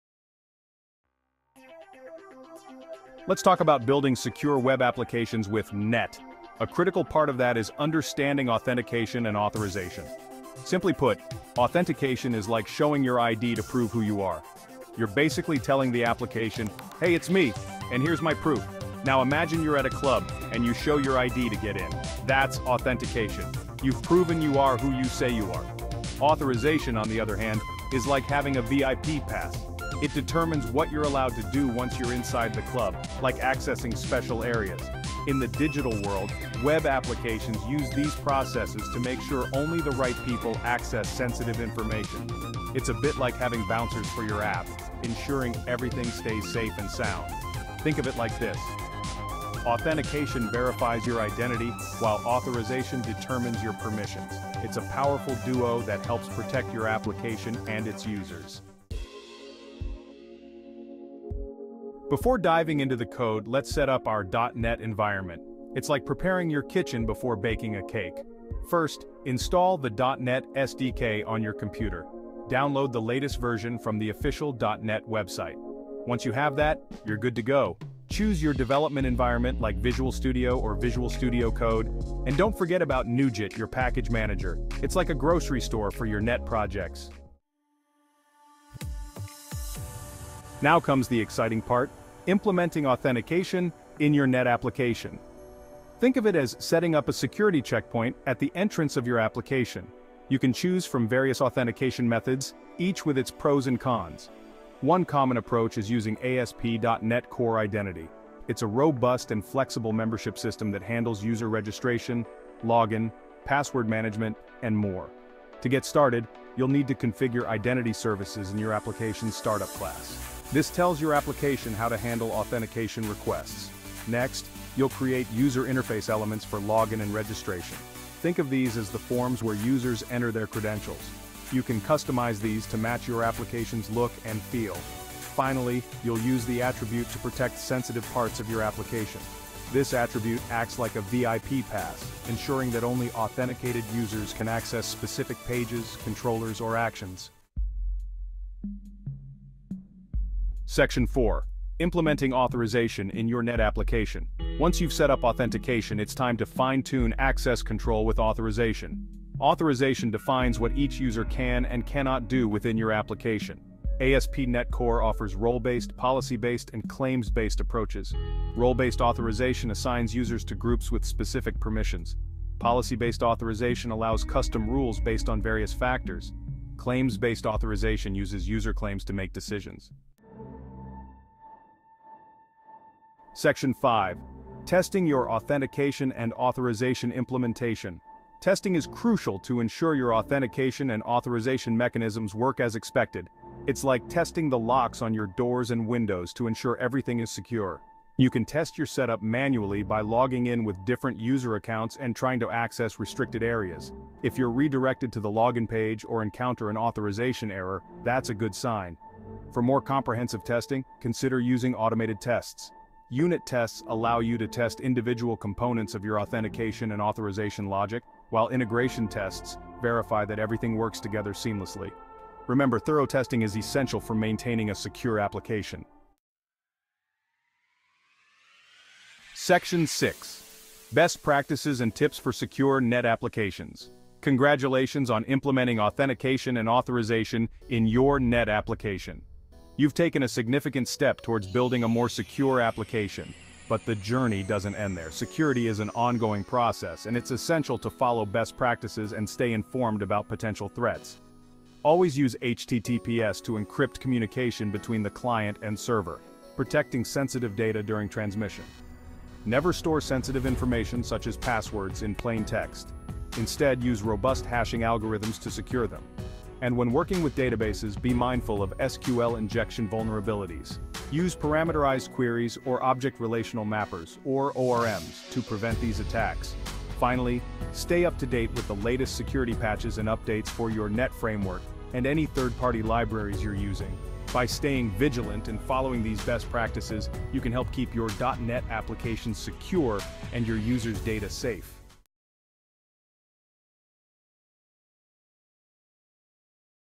Master Authentication & Authorization in web Applications Using.NET 🤩 Your Ultimate Roadmap 😤🎓
Dive into the essentials of Authentication and Authorization in .NET Applications🌐

1. Authentication - Think of it as showing your ID to verify who you are.
2. Authorization - Like a VIP pass that determines what you can access.

Timecodes:-
0:00 - Introduction to Authentication and Authorization
0:58 - Setting Up Your .NET Environment
1:27 - Implementing Authentication in Your .NET Application
2:27 - Implementing Authorization in Your .NET Application
3:00 - Testing Your Authentication and Authorization Implementation
3:46 - Best Practices and Tips for Secure .NET Applications